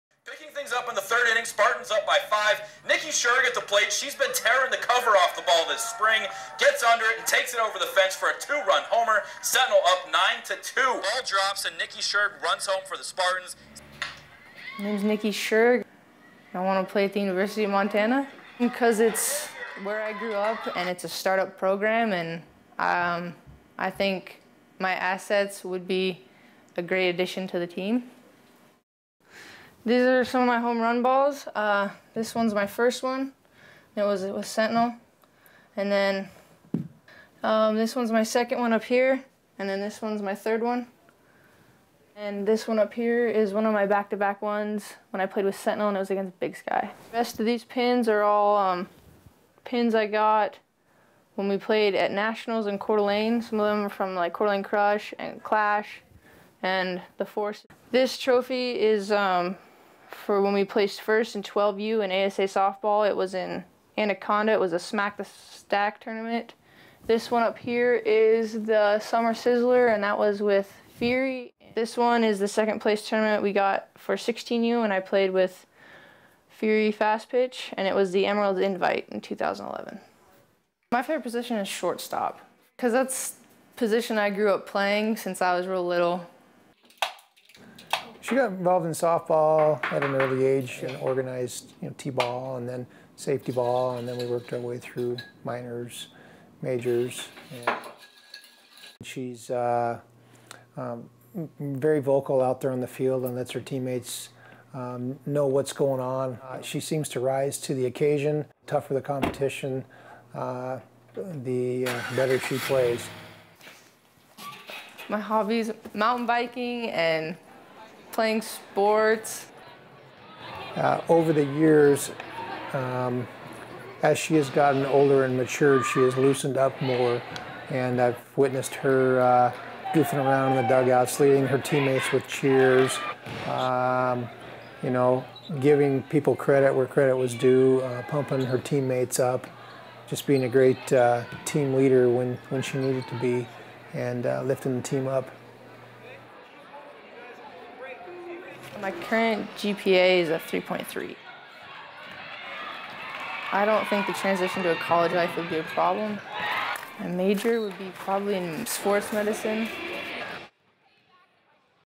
Nikki Shurg
A high school star pines for a collegiate career.